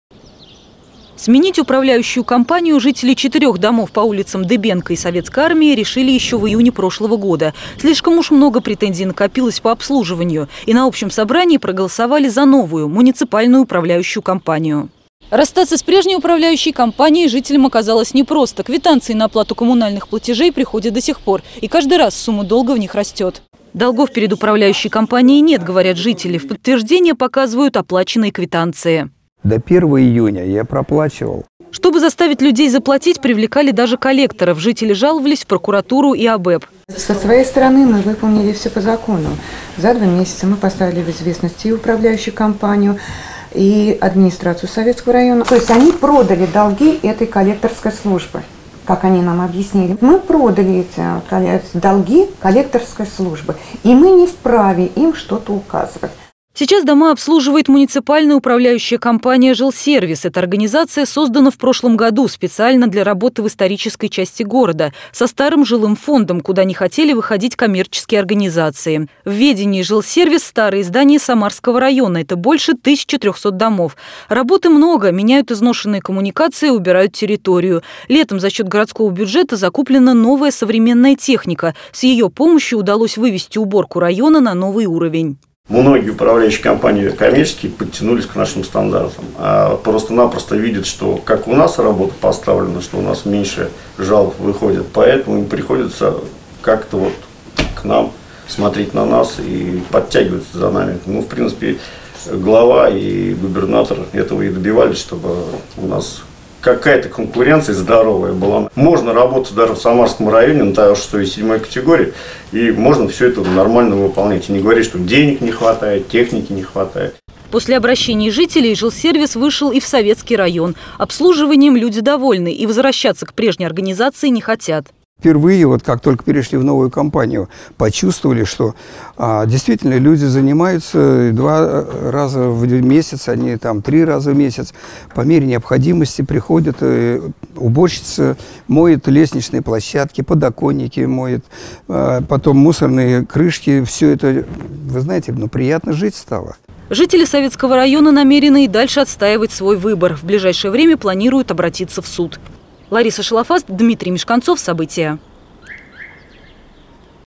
Чем грозит смена управляющей компании?
Коммунальным конфликтом обернулось желание жителей домов по улицам Дыбенко и Советской армии сменить управляющую компанию. С лета 2015 года перешли на обслуживание к муниципальной управляющей компании Жилсервис. Но прежняя организация с решением жильцов не согласна и продолжает выставлять счета за коммунальные услуги